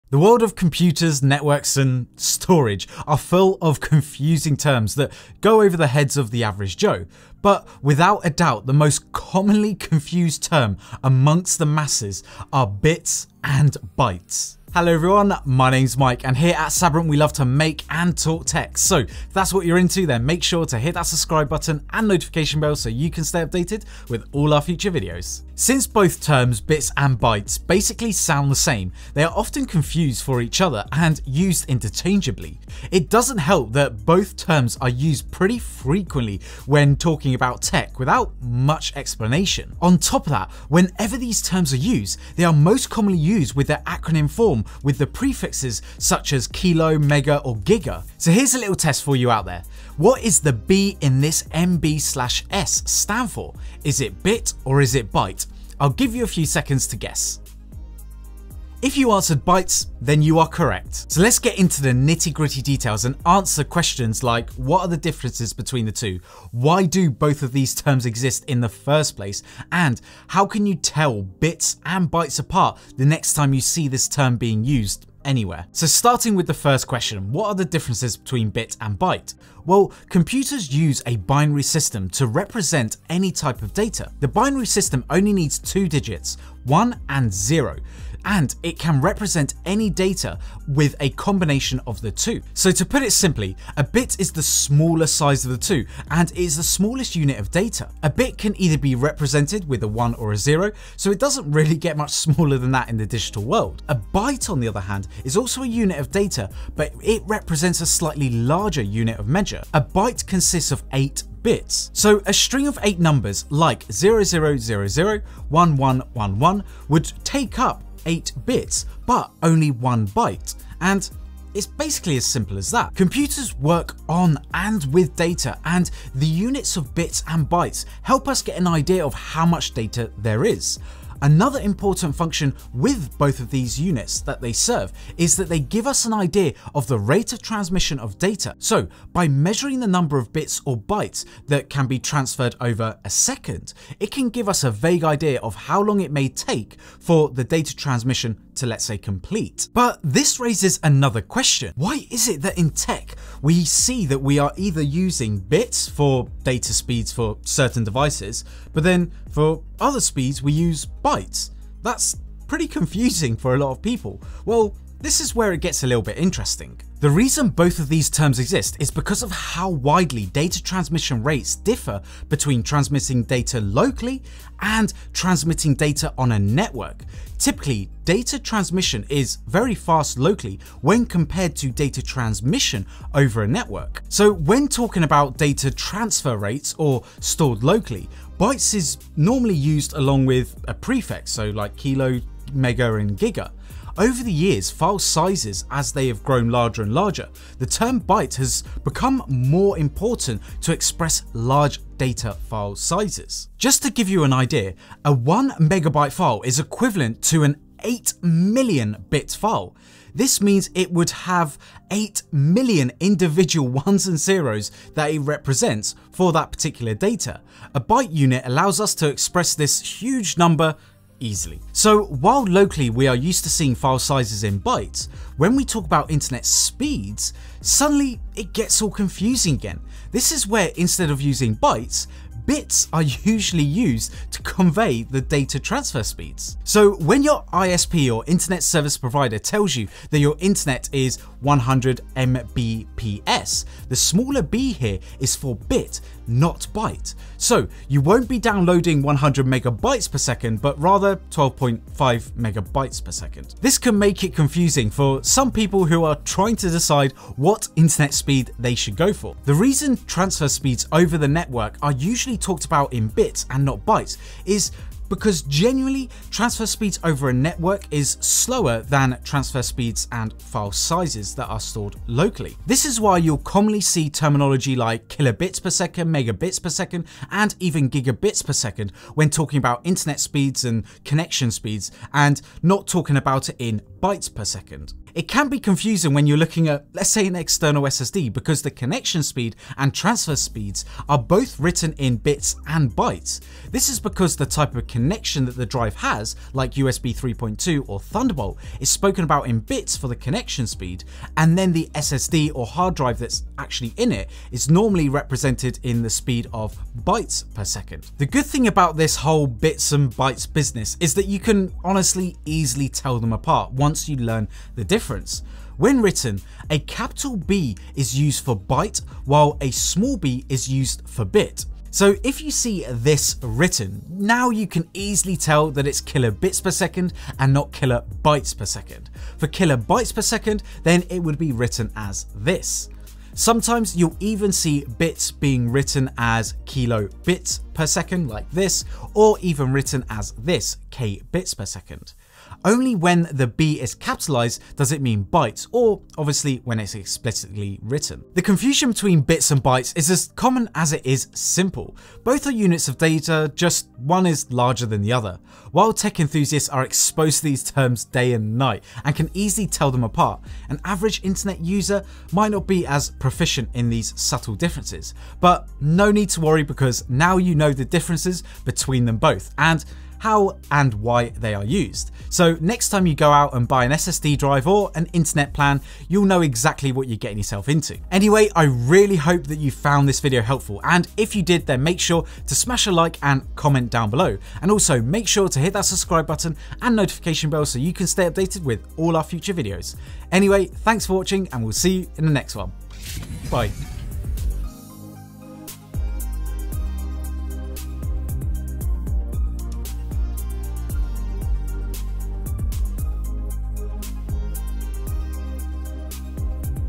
Bits And Bytes | What and Why?
bit and byte often get confused and even get misspoken many times for one another! The two represent units of data but what are the differences? Well in this video we break down when and how these are used!